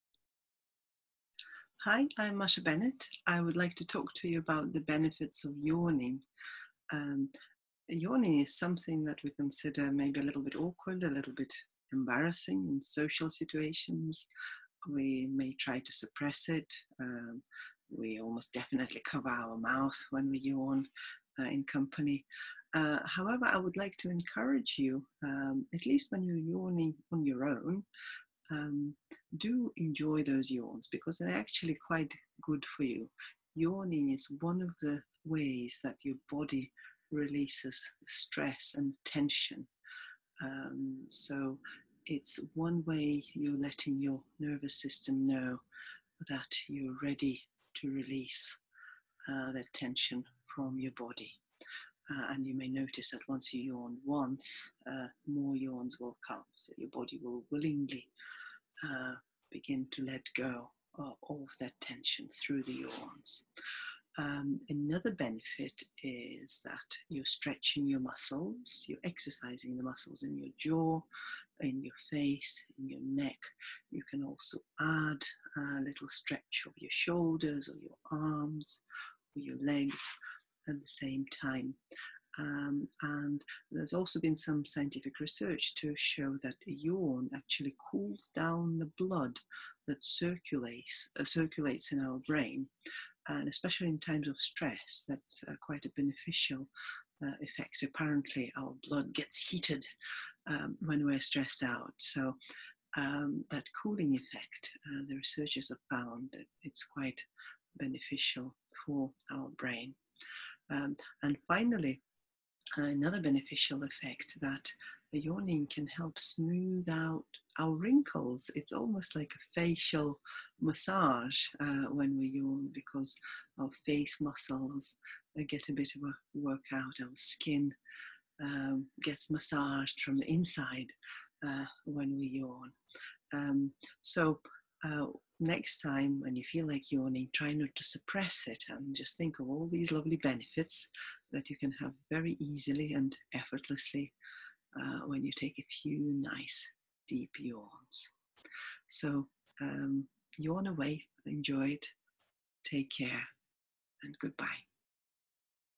Masha's Self-Help Toolbox: #15 About Yawning
Masha is presenting a series of short videos on simple and effective tools for self-help, emotional regulation and stress management.
Yawning helps to release stress and tension from the body, stretching and exercising muscles in the jaw, face and neck, cools down the blood which is said to have beneficial effect on the brain, and even helps to smooth out any wrinkles!

Masha Bennett is a registered psychotherapist, trainer and trauma specialist. Her websites are